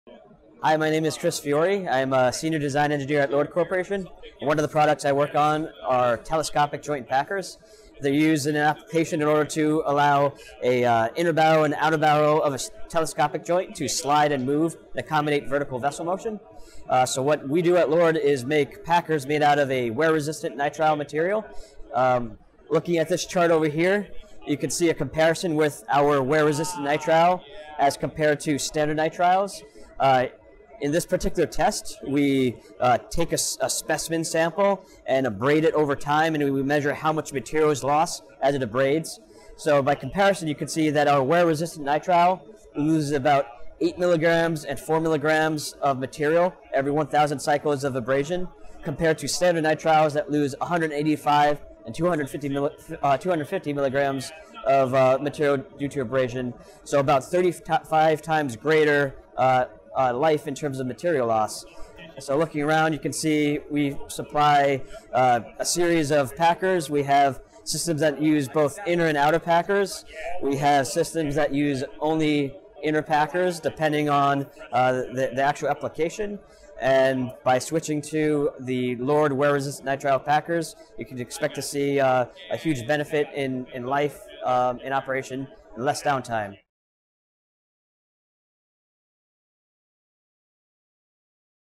Telescopic Joint Packer - Explained by Chris Fiore
Chris Fiore, a LORD engineer, explains our Telescopic Joint (TJ) Packer. It seals against the inner barrel of the slip joint. Our packers are long-lasting; with more reliable seals you can drill longer without interruption.